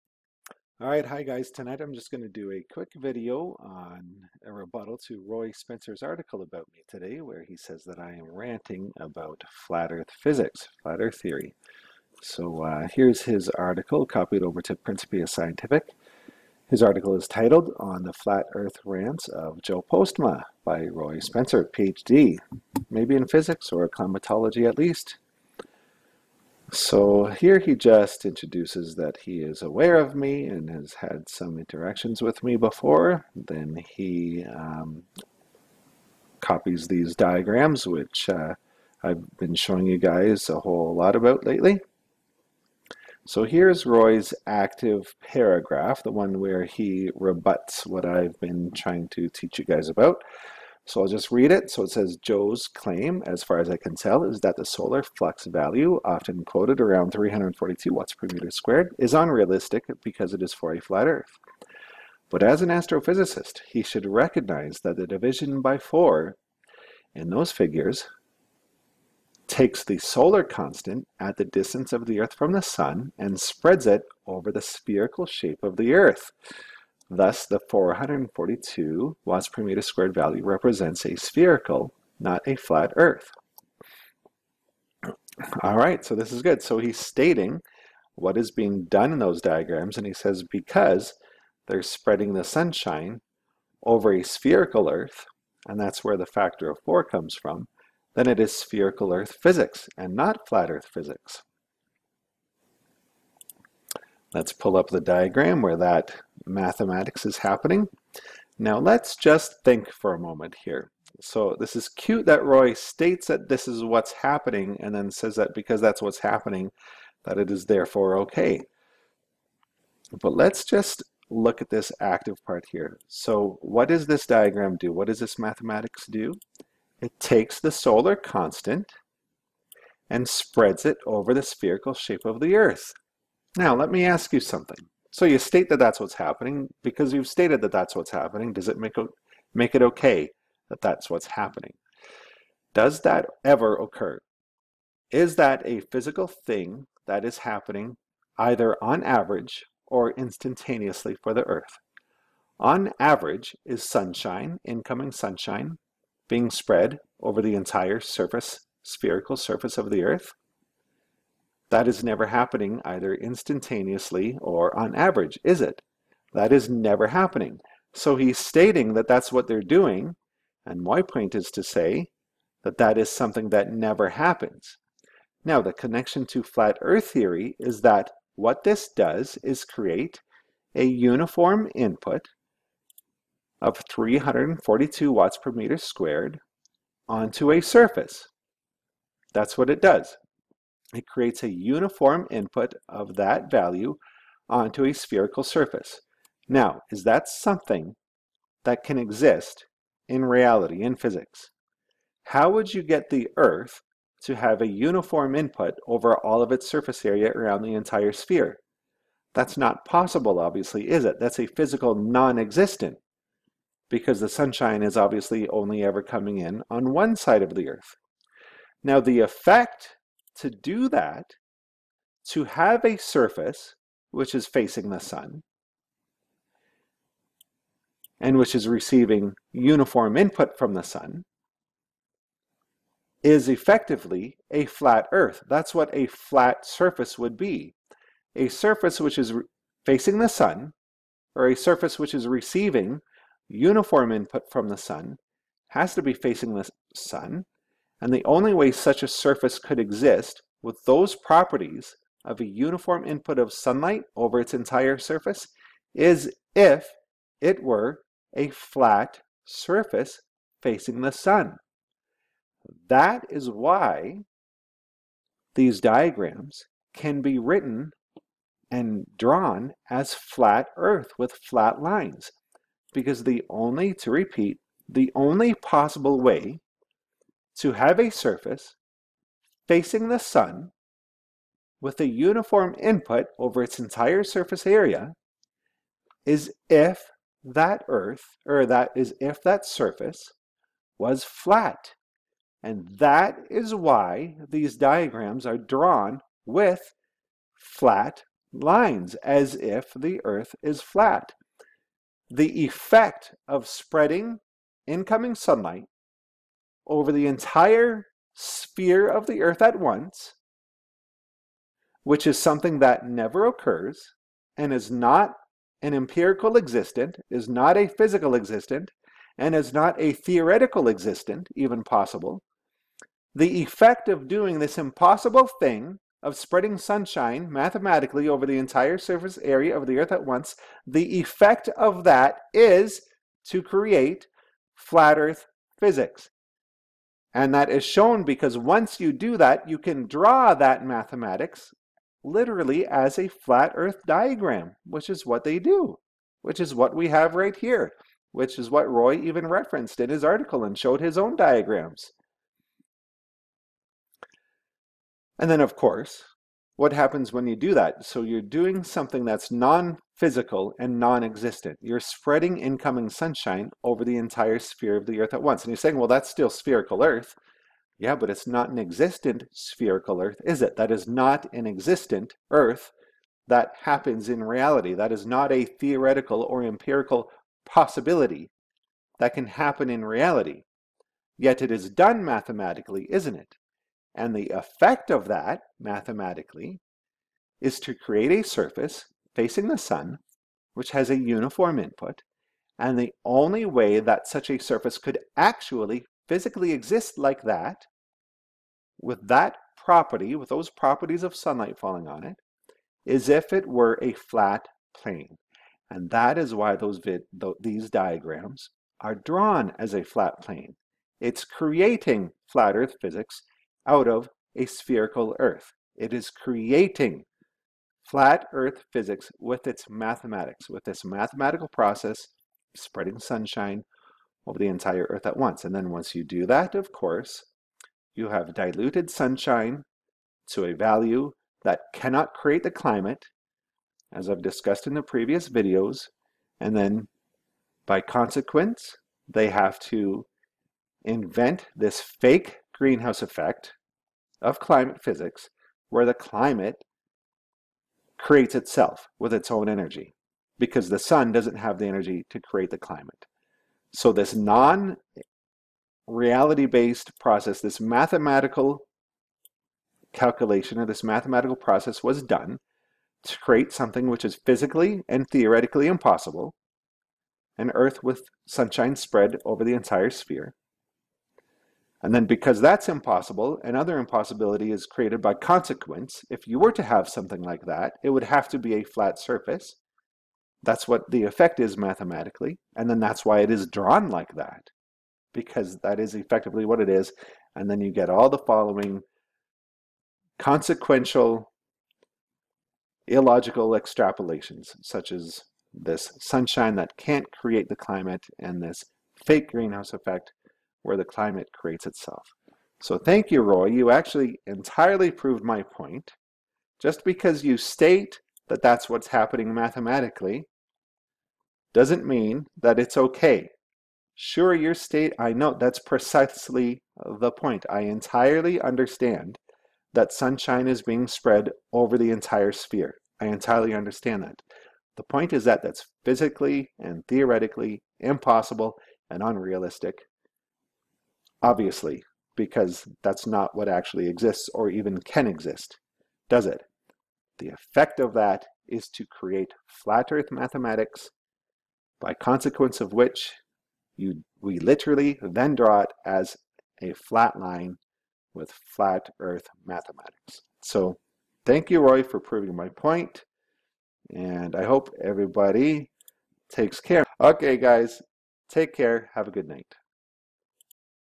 Response to Roy Spencer Regarding His Support of Flat Earth
Roy Spencer published an article today stating that I am incorrectly ranting about the fraud of flat Earth theory making its way into modern physics via climate science.  I analyze his statement and expose that climate science truly is indeed flat Earth theory: it is baked into the mathematics!